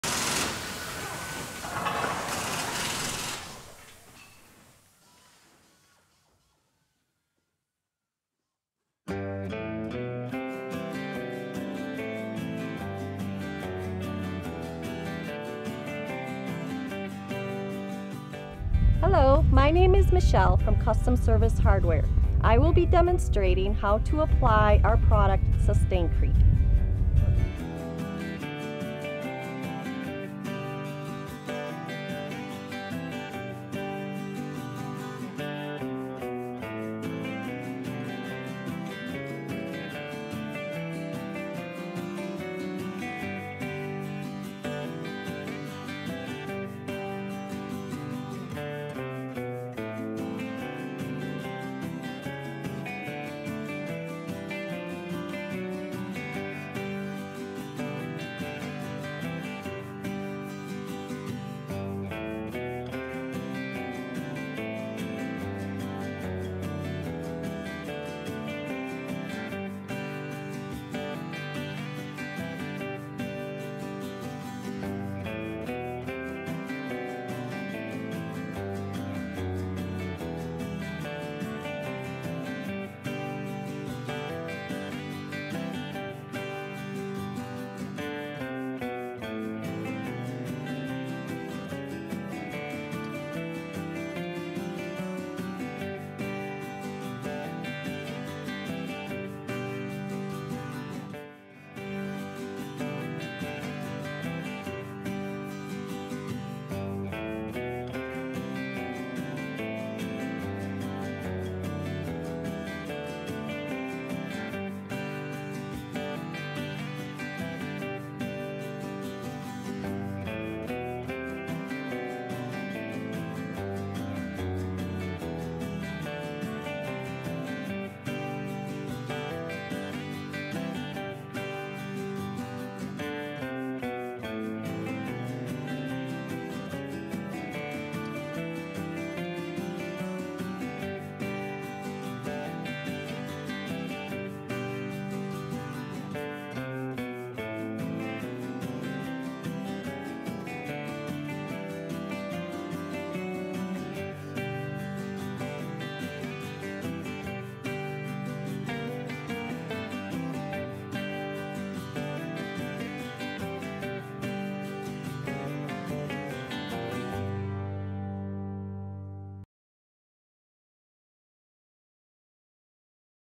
Sustain Krete | How to apply Sustain Krete
Sustain Krete is a soy-based sealant that blocks out water and salt, extending the life of your concrete.

This USA-made, soy-based penetrating concrete sealer is the result of a partnership between Purdue University and the Indiana Soybean Alliance. Sustain Krete provides superior protection against the harsh weather conditions throughout the country.